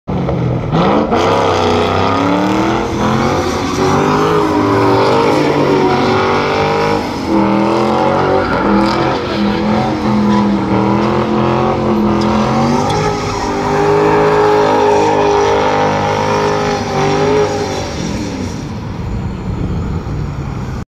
'07 Mustang GT donuts in the parking lot
no crowds were eaten